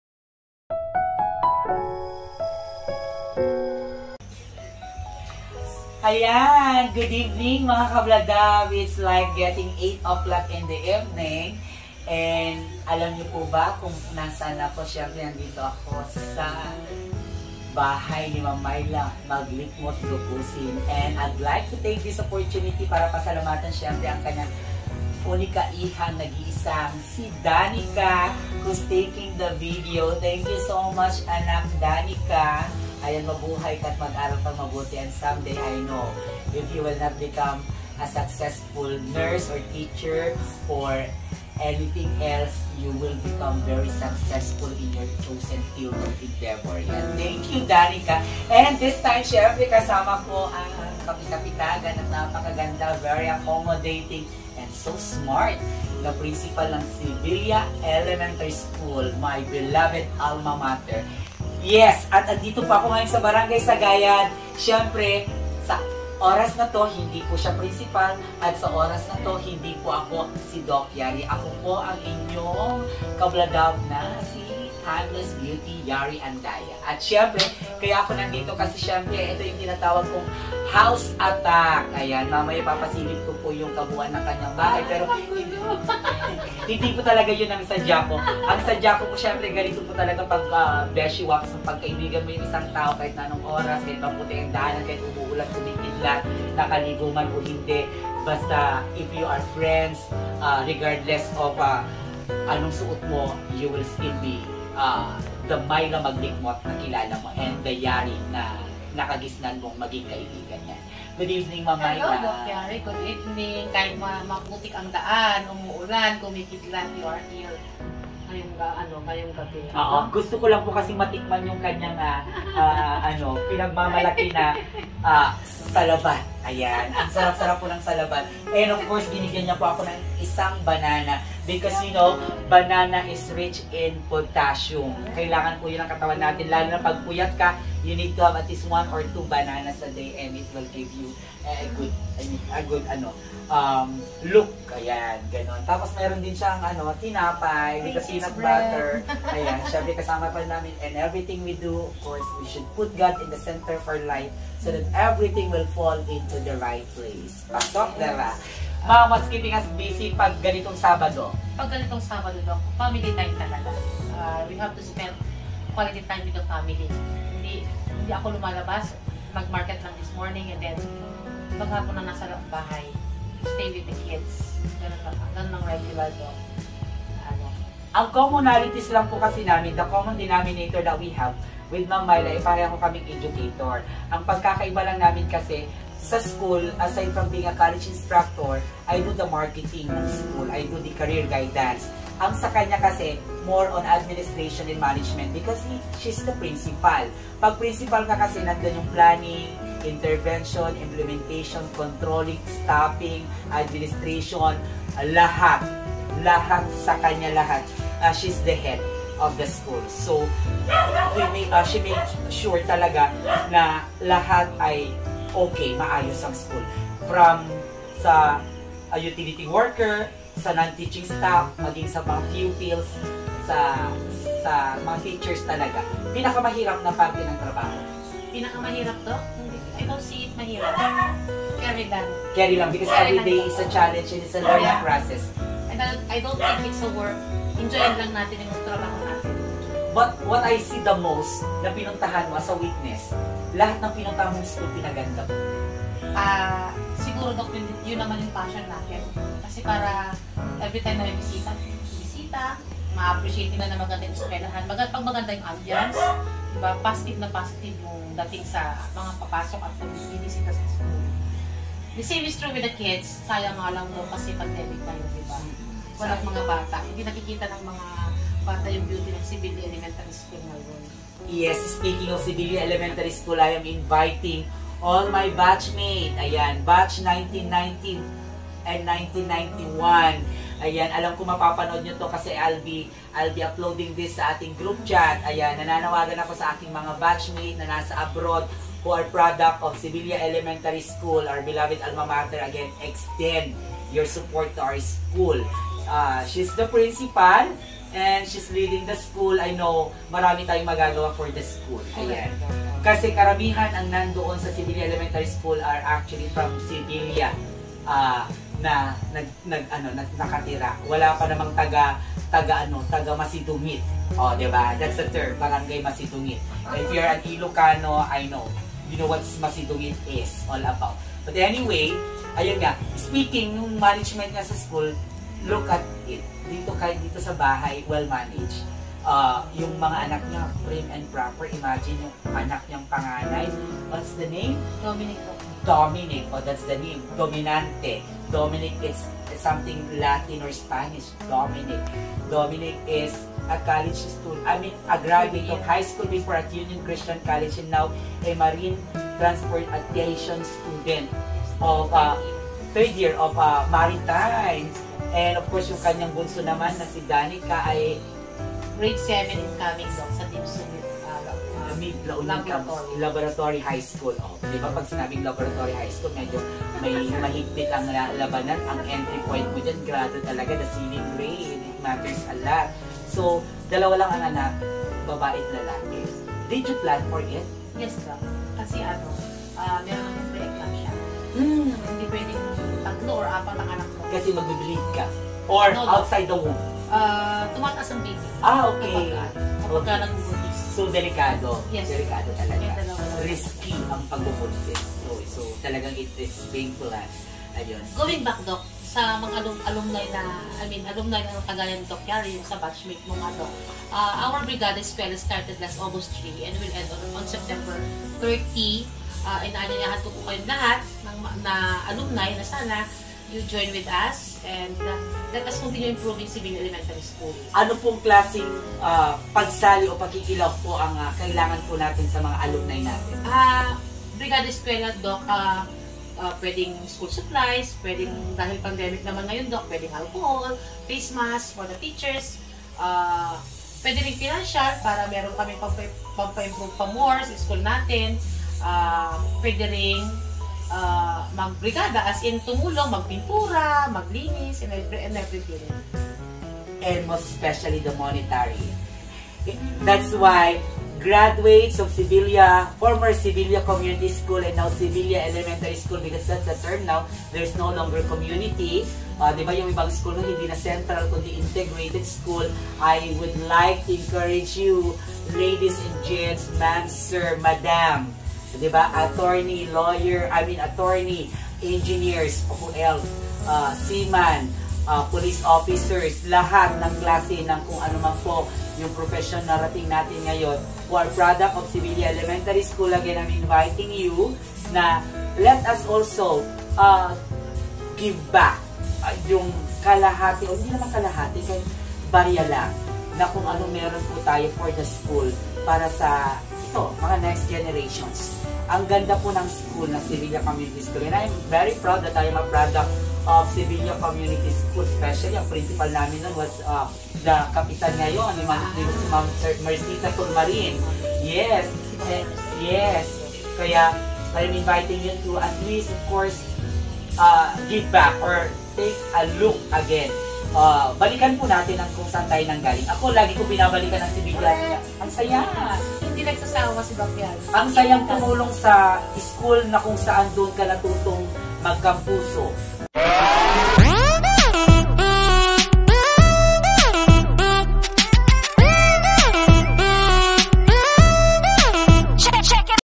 Part 1. Interview with the Principal of my Beloved Alma Mater Sevilla Community School.
Thank You Mam Myla Maglicmot Ducusin for sharing with me your precious time.